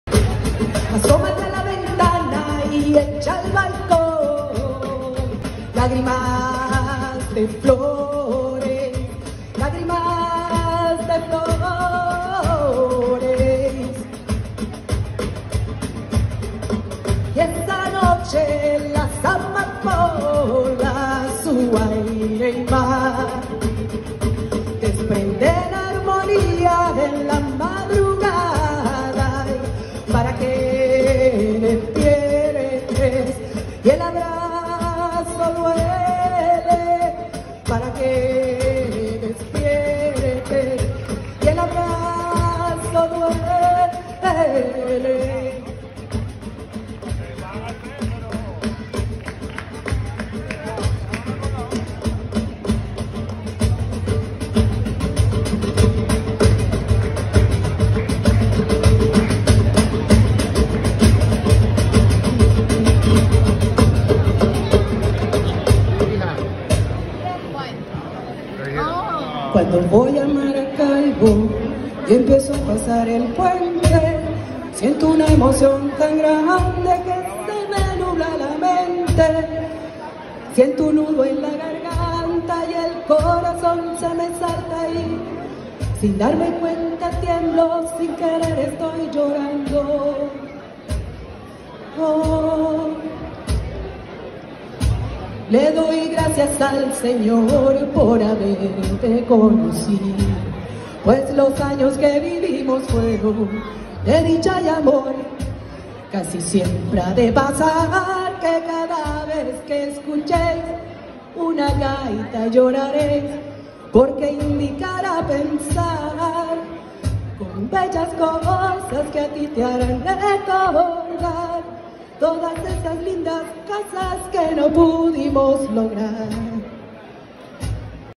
Fuego Flamenco en el Bazar y Parranda 2022
Bailando por Venezuela. Fusión de música flamenca con venezolana.